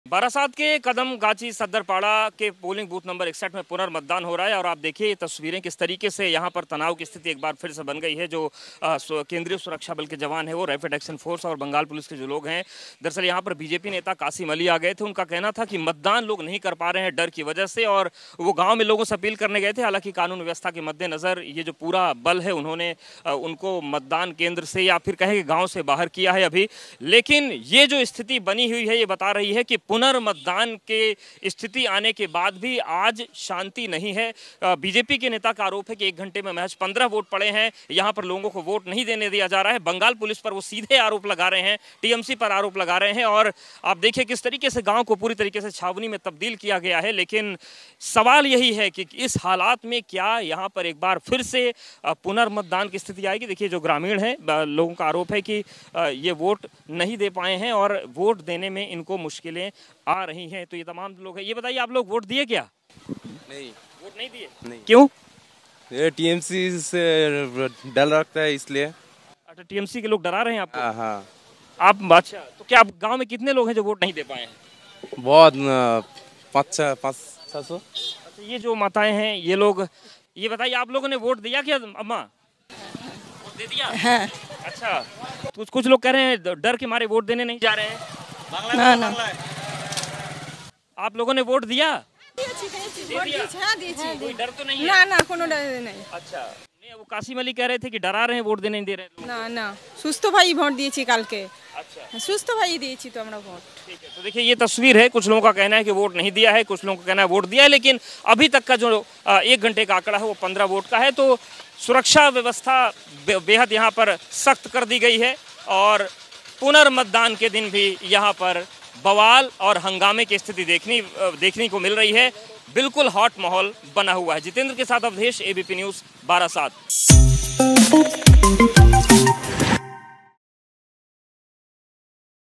Lok sabha Election 2024: बारासात में एक घंटे में पड़े 15 वोट, डर के घरों में बैठे हैं वोटर
भाजपा नेता कासिम अली गांव में जाकर लोगों से ना डरने की और वोट करने की अपील कर रहे थे पुलिस और सुरक्षाबल ने गांव से बाहर किया | बारासात में एक घंटे में पड़े 15
वोट, डर के घरों में बैठे हैं वोटर